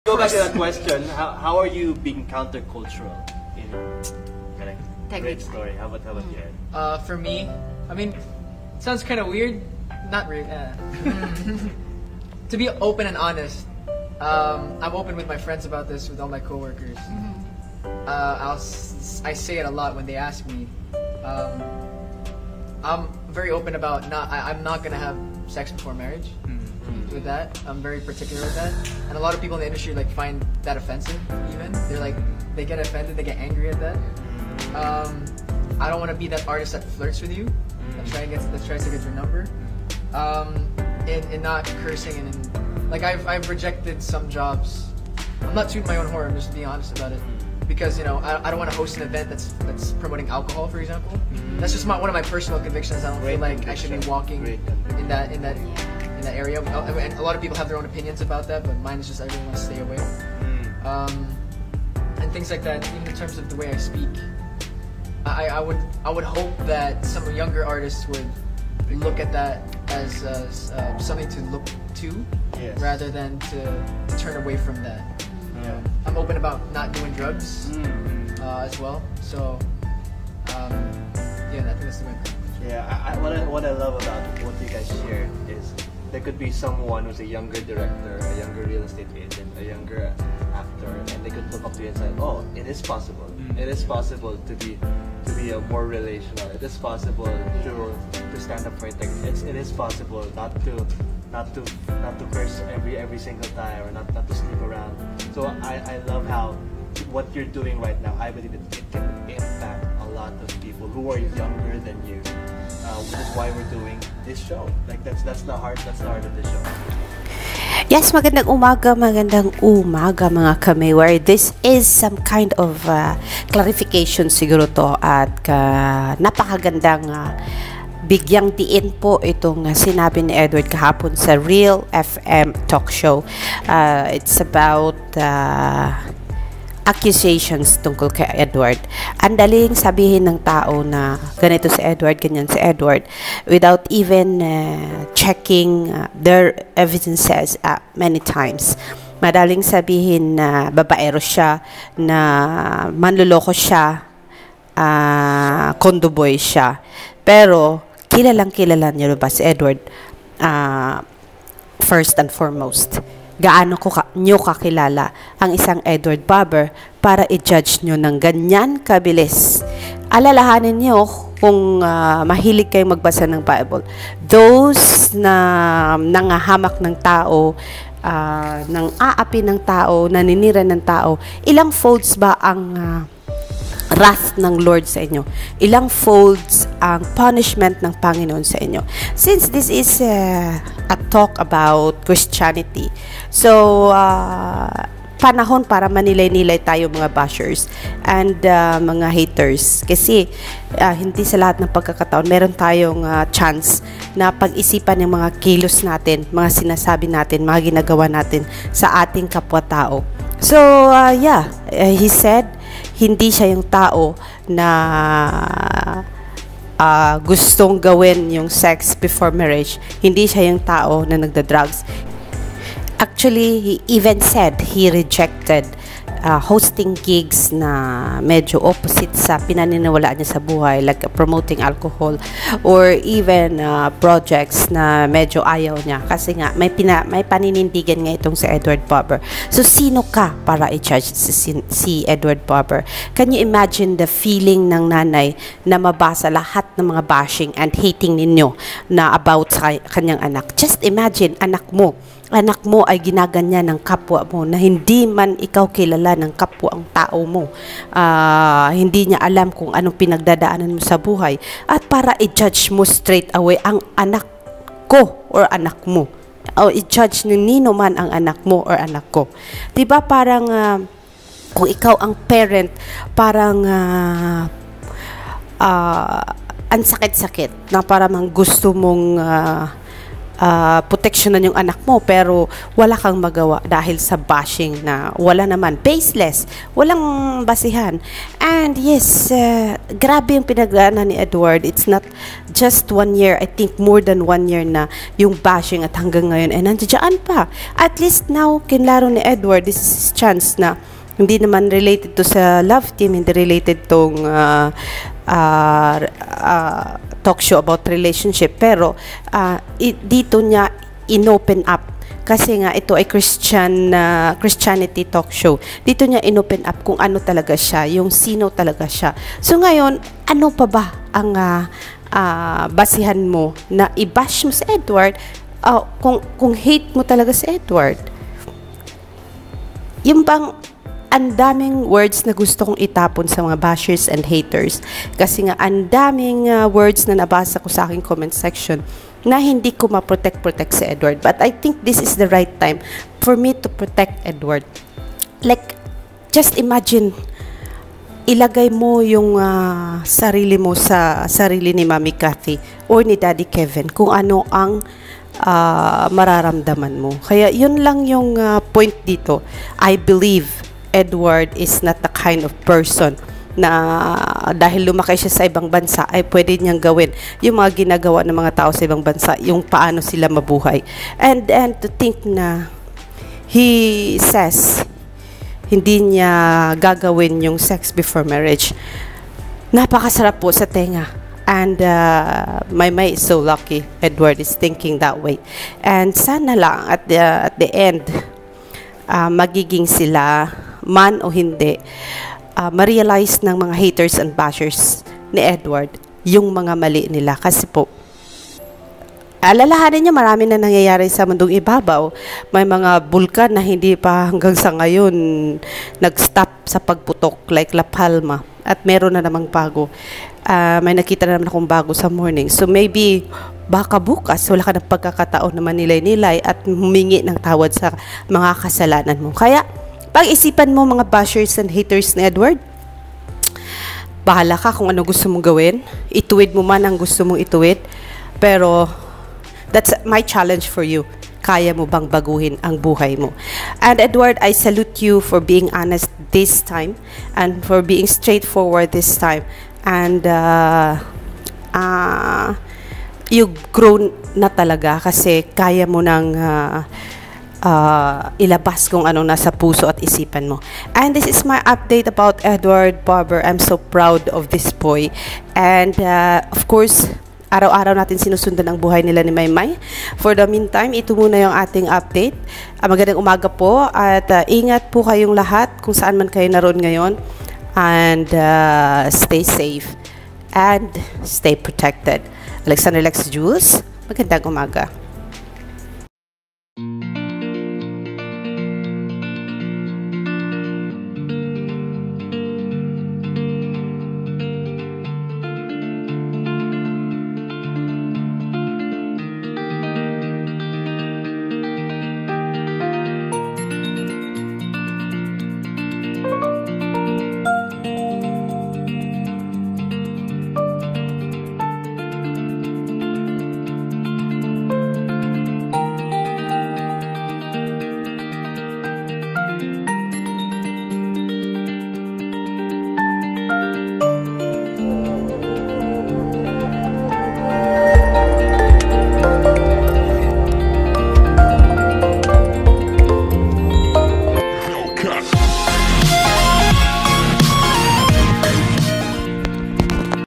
Edward Barber said, “I do not want to have sex before marriage” May Ibabash ka pa ba kay Edward?
Ayaw mo mang maniwala kasi hate mo si Edward  bahala ka kawalan mk yan kasi minus points ka kay God  naku baka maka recieve ka ng punishment kay God 7 folds.